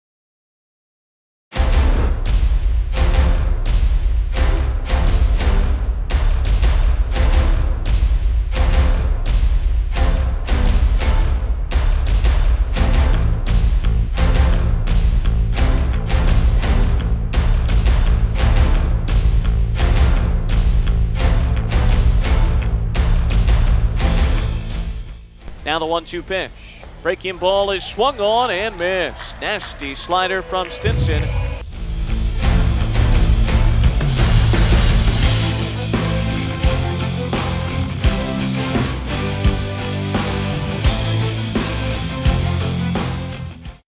B-Mets Most Vaulable Picther Award Video 2010
Created for B-Mets end of the year awards in 2010. For Most Valuable Pitcher Josh Stinson. Edited from 2010 highlights.